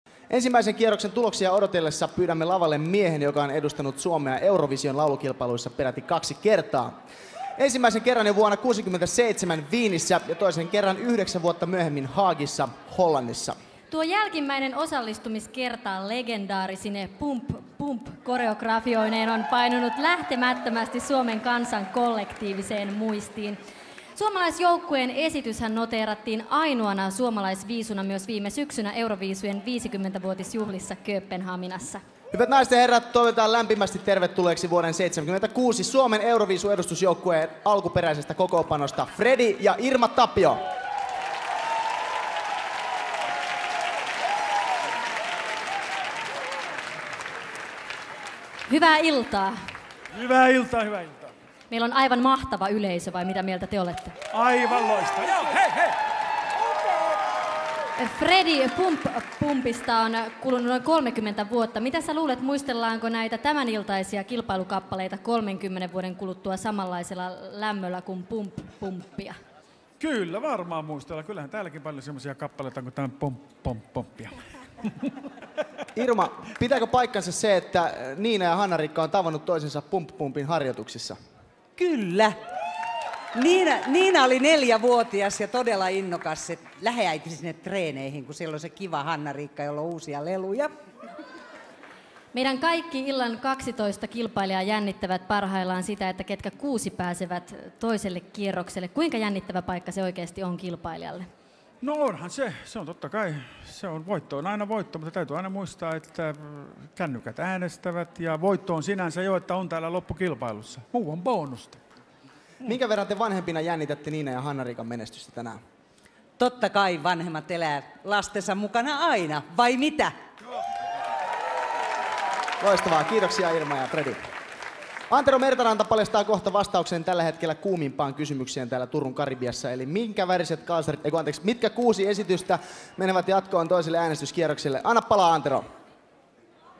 Fredin ja Irma Tapion haastattelu (2006)
Vuosien 1967 ja 1976 euroviisuedustaja Fredi ja vuonna 1976 Ystävät-yhtyeessä edustanut Irma Tapio esiintyivät Suomen 40. euroviisukarsinnoissa juhlavuoden kunniaksi.